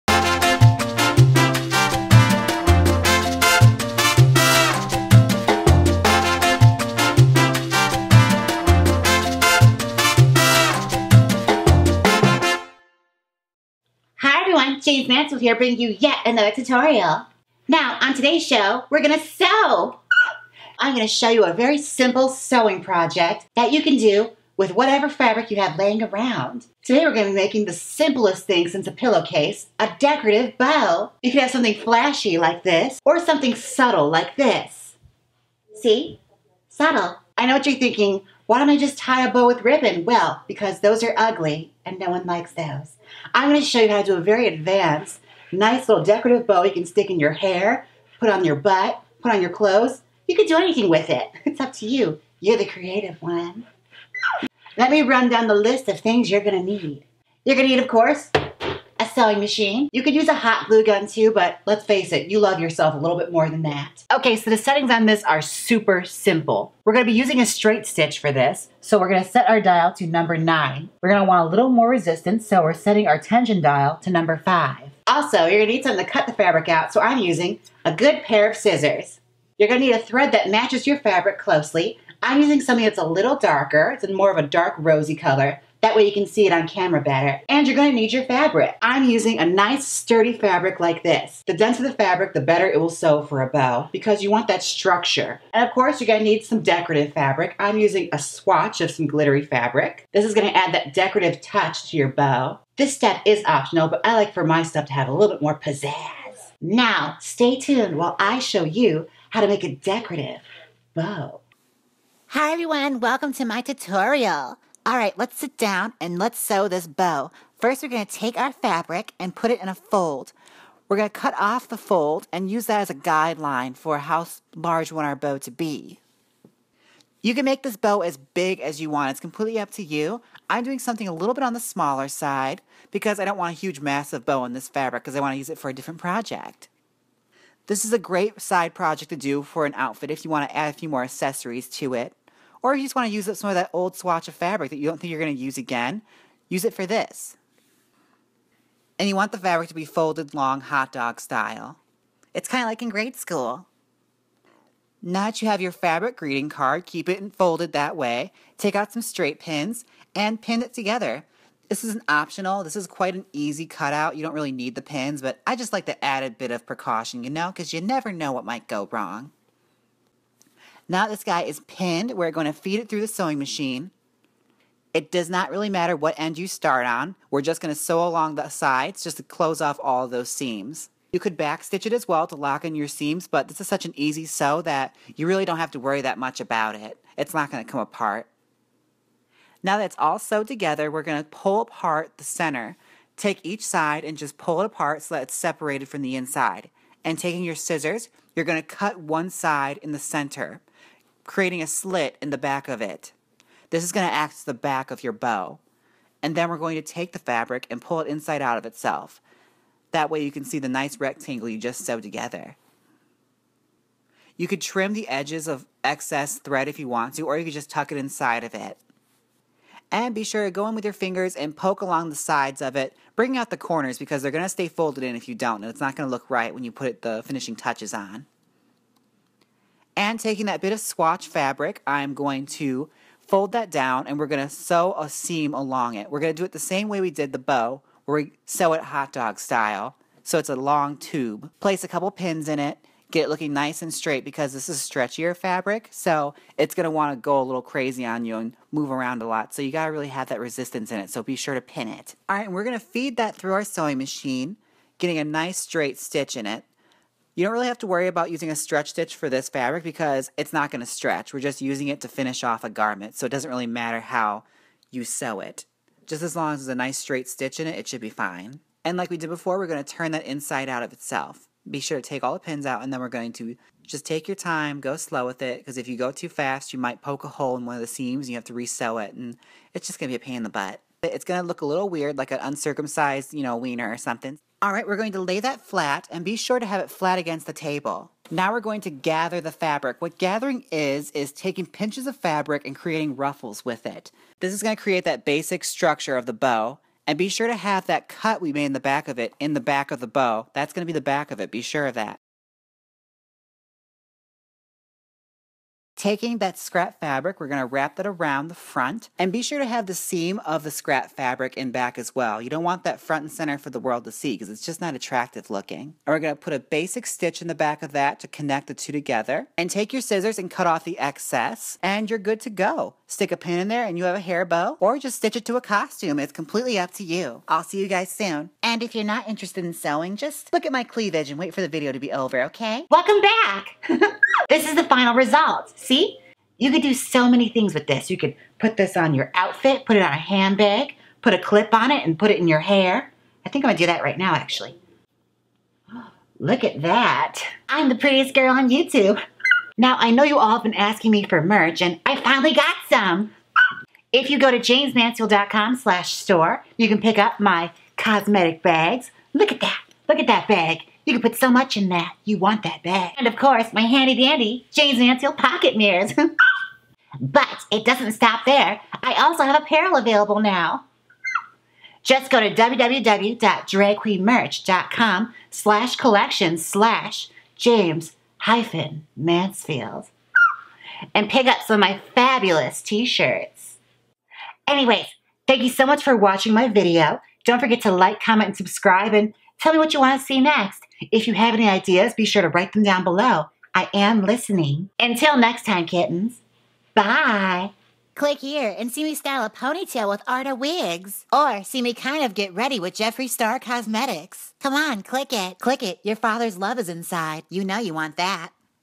HOW TO MAKE A BOW | EASY SEWING TUTORIAL | DRAG QUEEN COSTUMES | JAYMES MANSFIELD
On this weeks show we are going back to basics. I wanted to give a smaller more bite-sized lesson this week so I'm showing you something way simpler than my last sewing tutorial. It's a great introduction to sewing for those who just want to get a grasp on how a sewing machine works. You can make this in five minutes, and use it to embellish your costumes in countless ways.